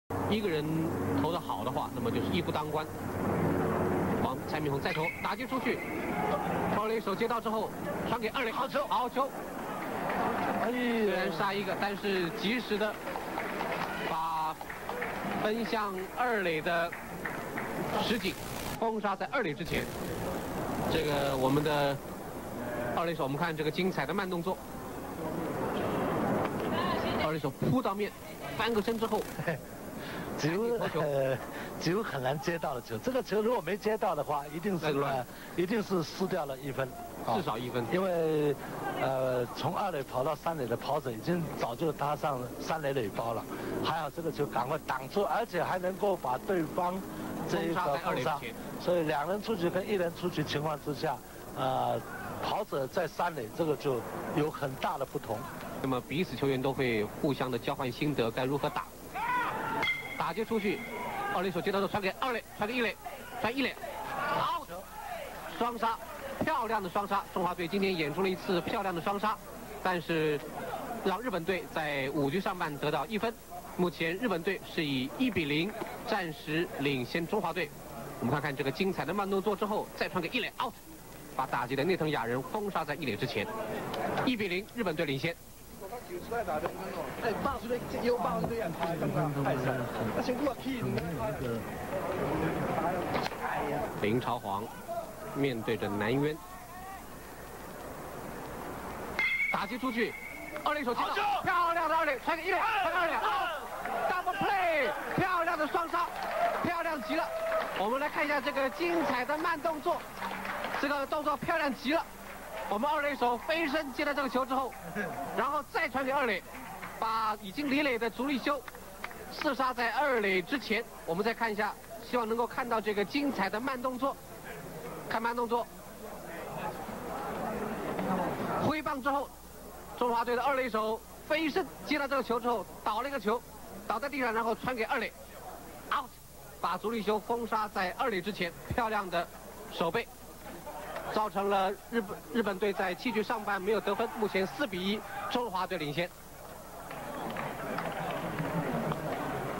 棒球影音館 "二壘魔術師" 黃忠義 (1989 亞錦賽 中華 vs 日本)
中華隊"二壘魔術師" 黃忠義展現職業水準, 履履救下險球.